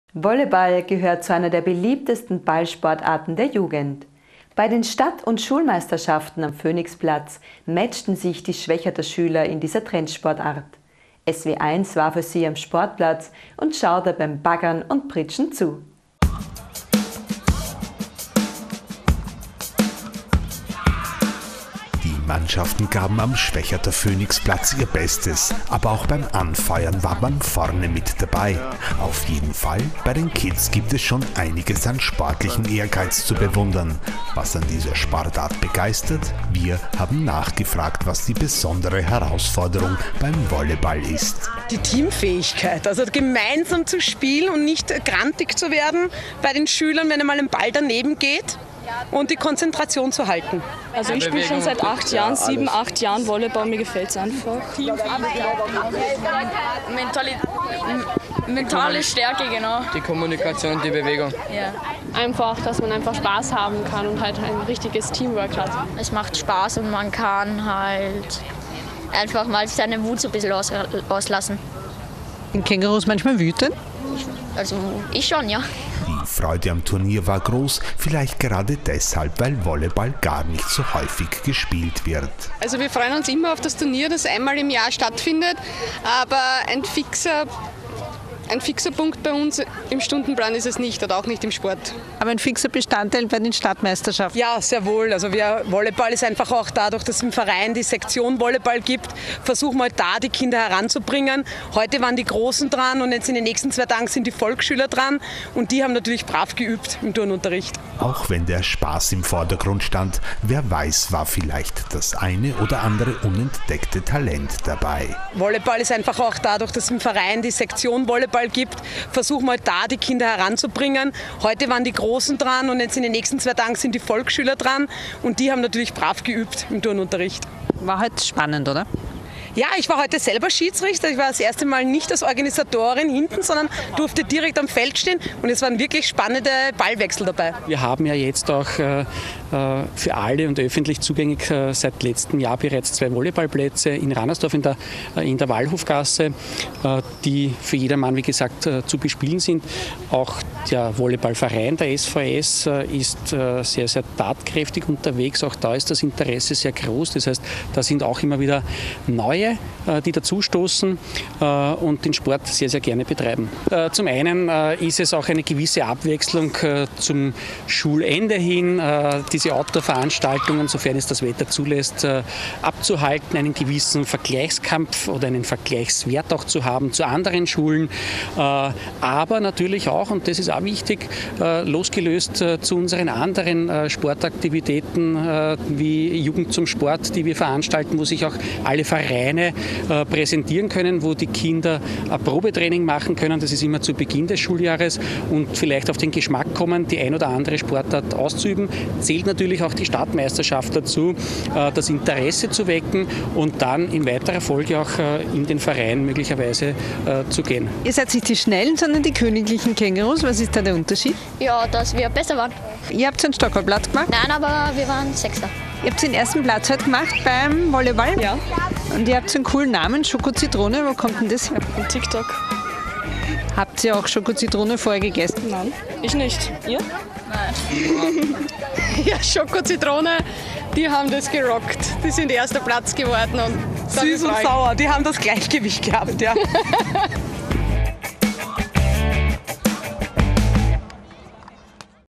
Volleyball Schulmeisterschaften in Schwechat
Volleyball gehört zu einer der beliebtesten Ballsportarten der Jugend. Bei den Stadt- und Schulmeisterschaften am Phoenixplatz matchten sich die Schwechater Schüler in dieser Trendsportart. SW1 war für Sie am Sportplatz und schaute beim Baggern und Pritschten zu.